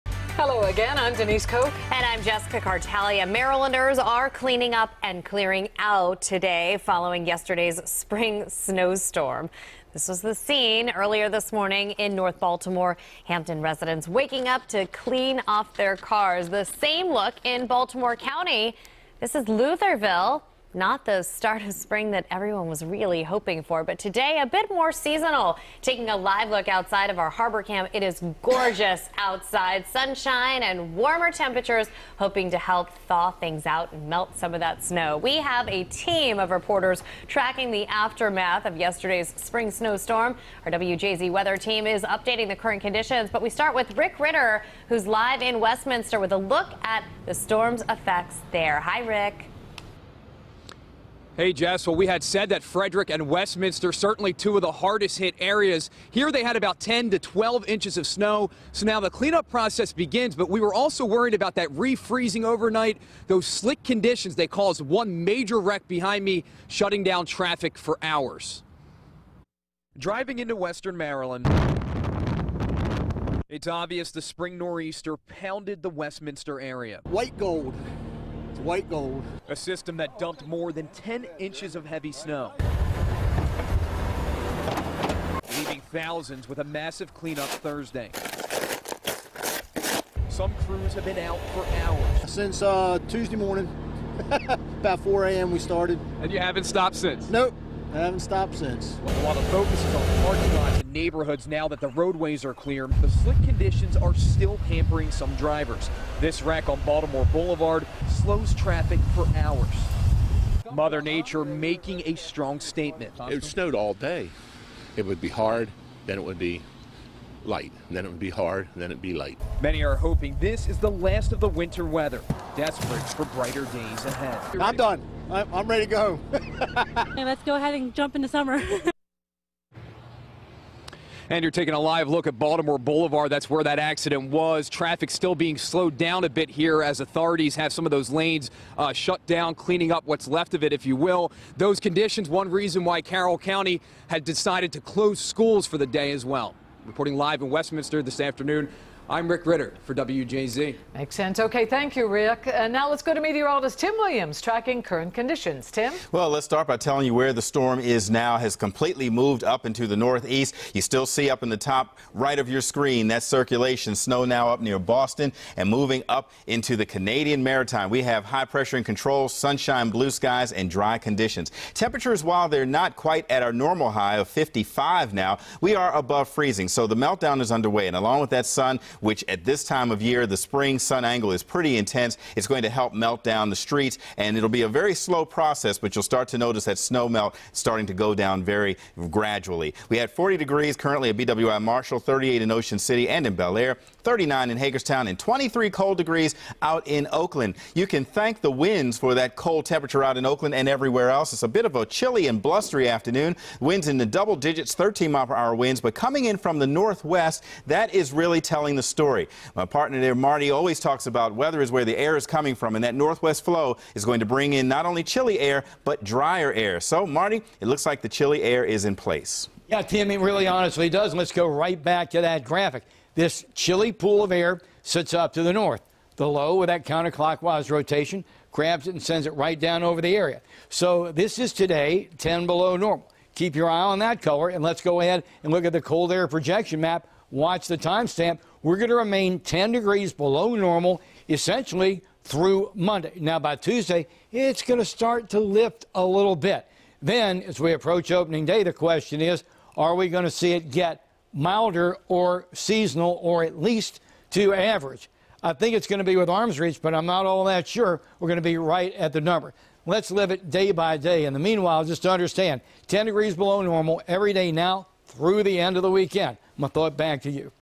Marylanders Cleaning Up, Digging Out After Snowfall In First Days Of Spring
Rick Ritter reports from one of the hardest hit areas of Maryland, and Tim and Marty have your forecast for the rest of the week.